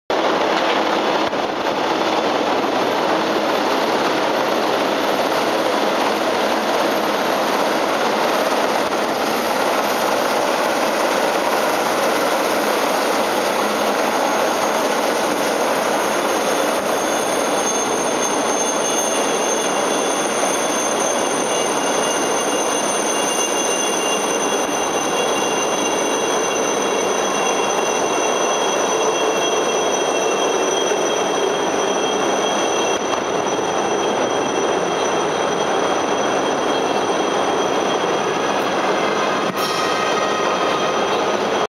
Lintas Hormat Di-Raja Unit Udara Angkatan Tentera Malaysia
Lintas Hormat Di-Raja Unit Udara Angkatan Tentera Malaysia Sempena Istiadat Mengarak Panji-Panji Yang Di-Pertuan Agong Sempena Keputeraan
Seri Paduka Baginda Yang Di-Pertuan Agong Di Dataran Merdeka Pada 4 Jun 2011. Malaysian Supreme Ruler Seri Paduka Baginda Yang Di-Pertuan Agong. Daulat Tuanku